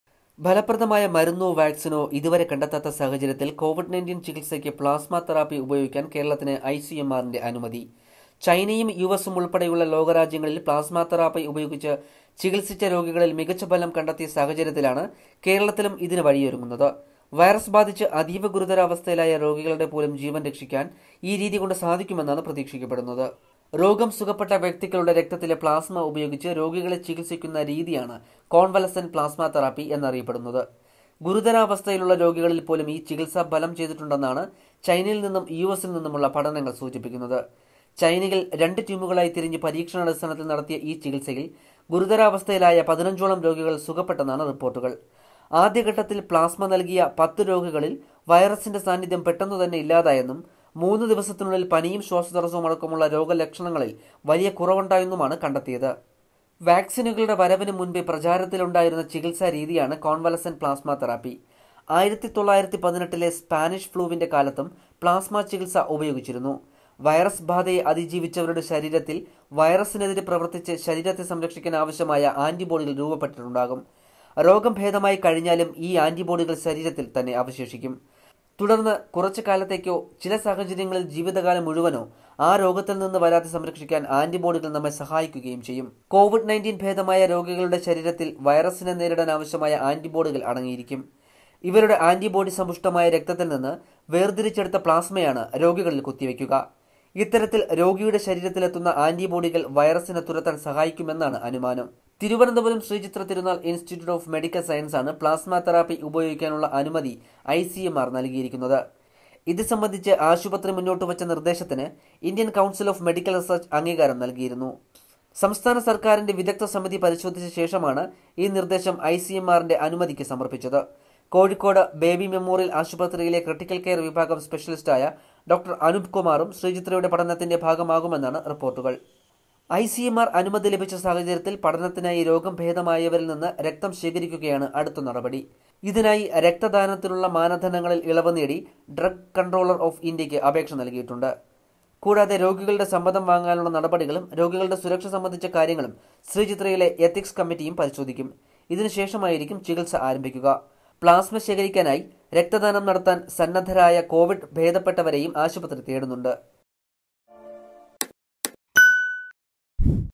'കേരളത്തിലും പ്ലാസ്മ തെറാപ്പി' എന്താണ് പ്ലാസ്മ തെറാപ്പി | Kerala | Samayam Malayalam
ചൈനയും യുഎസും ഉള്‍പ്പെടെയുള്ള ലോകരാജ്യങ്ങളില്‍ പ്ലാസ്മ തെറാപ്പി ഉപയോഗിച്ച് ചികിത്സിച്ച രോഗികളില്‍ മികച്ച ഫലം കണ്ടെത്തിയ സാഹചര്യത്തിലാണ് കേരളത്തിലും ഇതിന് വഴിയൊരുങ്ങുന്നത്. വൈറസ് ബാധിച്ച് അതീവഗുരുതരാവസ്ഥയിലായ രോഗികളുടെ പോലും ജീവൻ രക്ഷിക്കാൻ ഈ രീതി കൊണ്ട് സാധിക്കുമെന്നാണ് പ്രതീക്ഷിക്കപ്പെടുന്നത്.